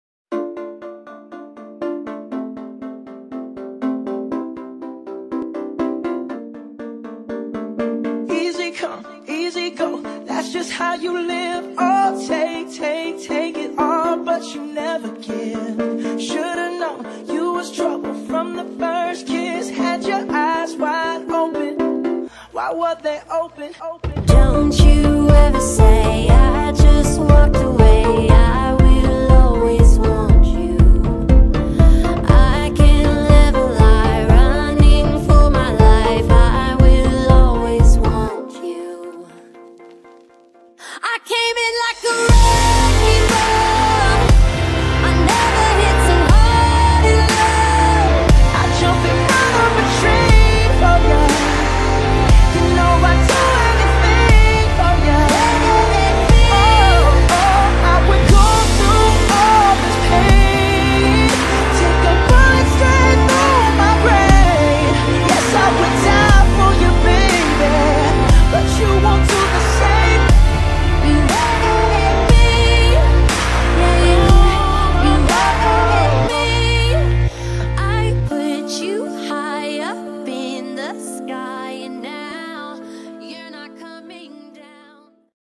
Unfinished Business Trailer
Watch the exclusive new trailer for Unfinished Business by Isabelle Drake, the newest addition to Totally Bound's What's her Secret? imprint.